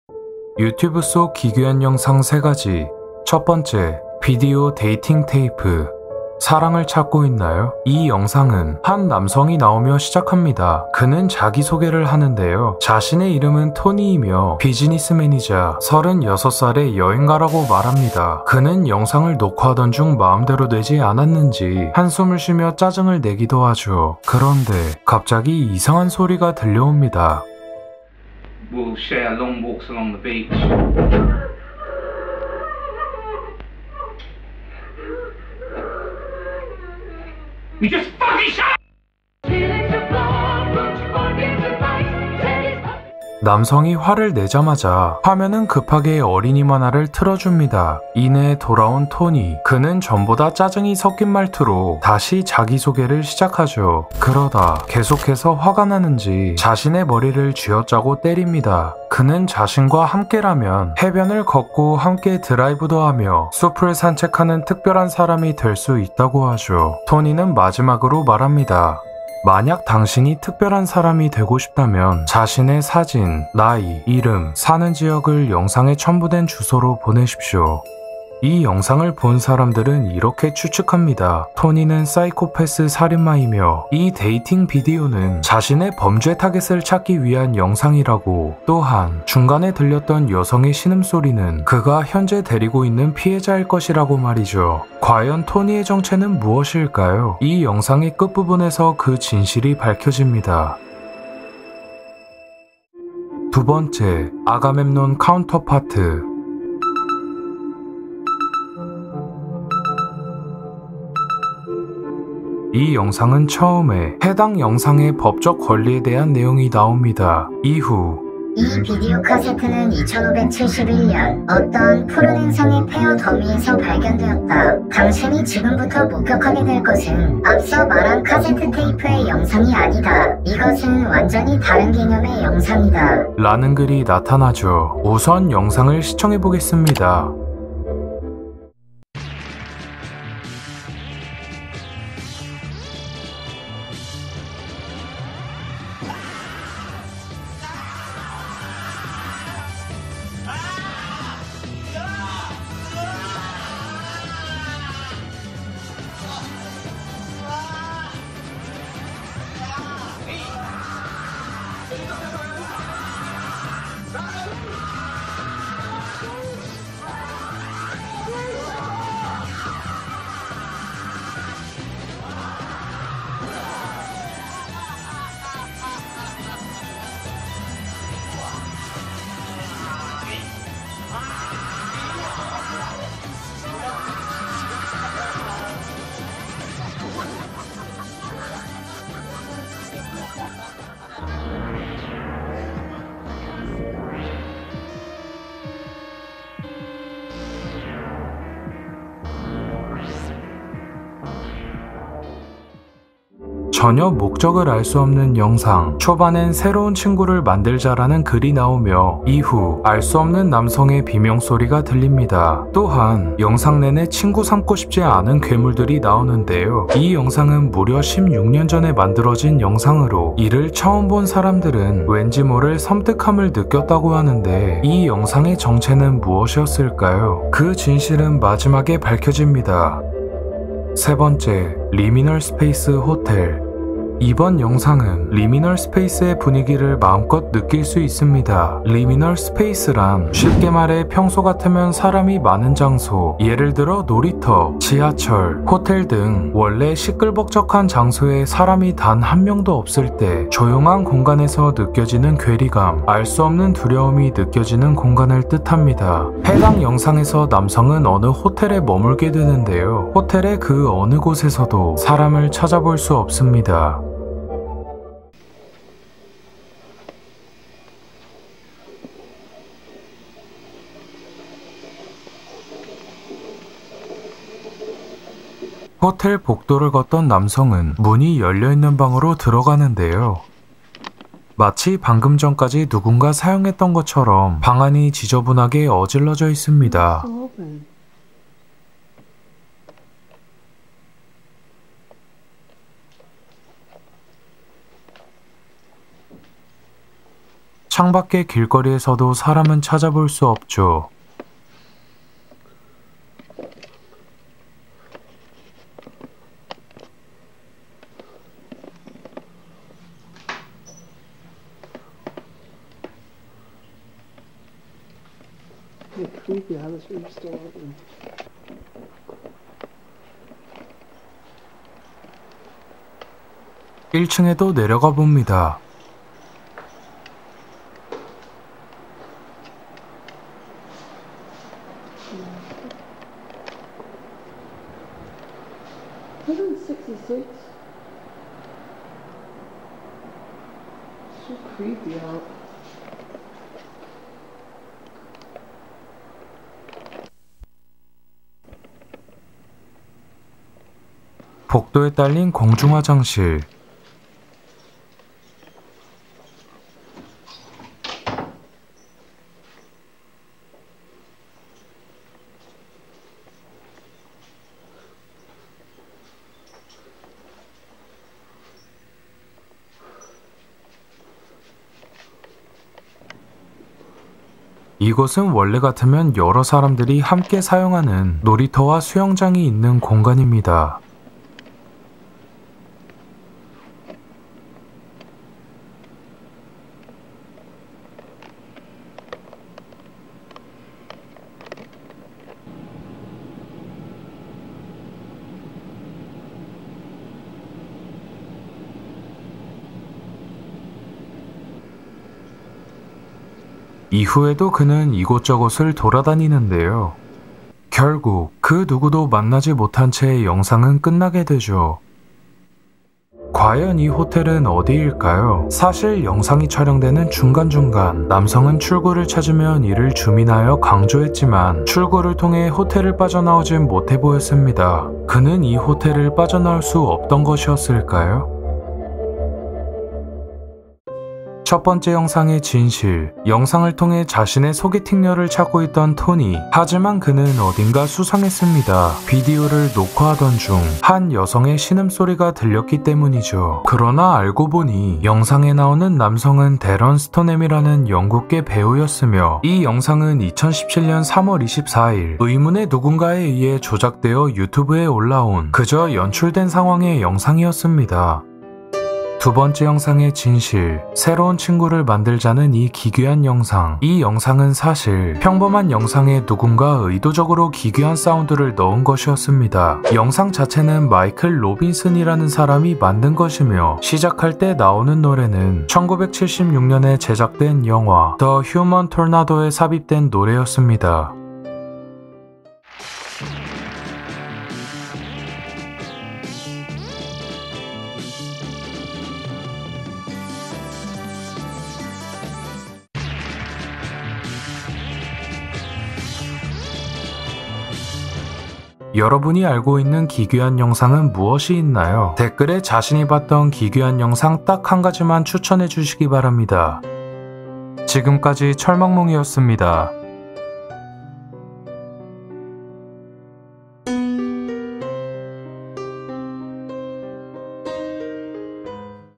폐가에서 발견된 비디오를 켜보니 충격적인 영상이 재생되었다
오늘은 유튜브에 올라온 기괴한 영상들에 대해 알아봤습니다.

세 가지 영상 모두 제가 재밌게 봤던 것들인데요

여러분도 재밌으면 좋겠습니다.